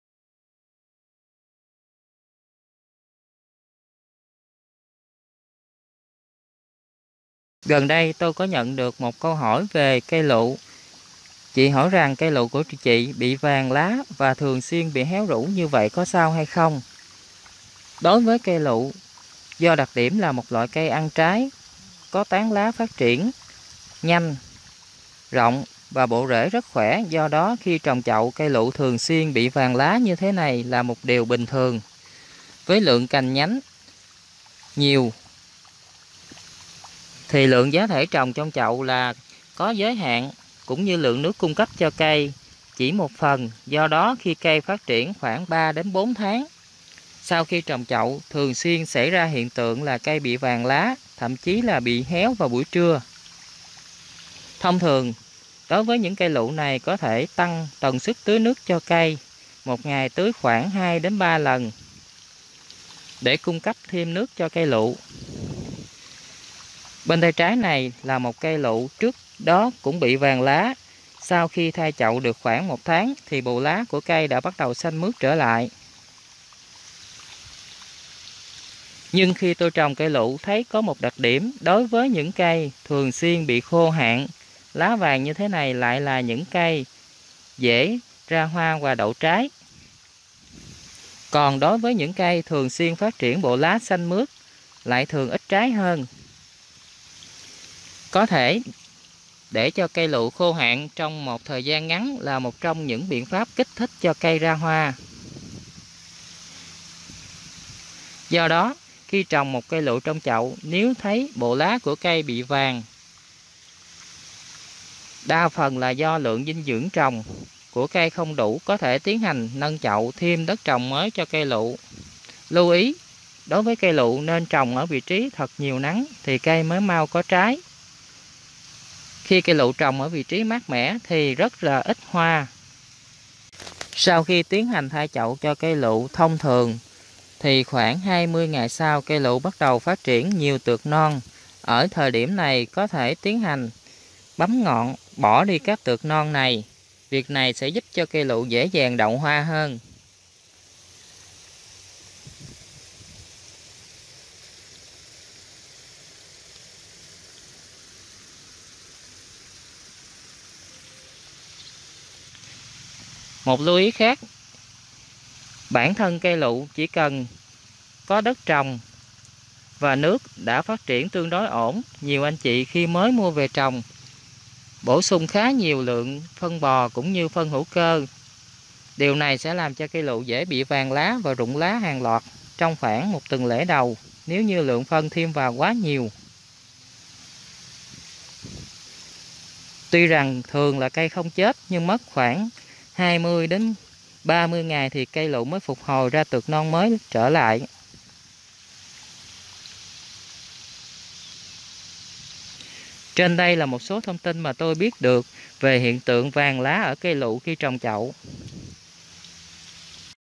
Trồng cây lựu trong chậu bị vàng lá và héo rũ nguyên nhân do đâu?
Trồng cây lựu bị vàng lá và héo rũ nguyên nhân do đâu?
Thông thường cây lựu khi trồng chậu thường bị vàng lá và héo rũ sau vài tháng trồng, nguyên nhân thường do đất trồng không đủ dinh dưỡng và nước cung cấp cho cây lựu.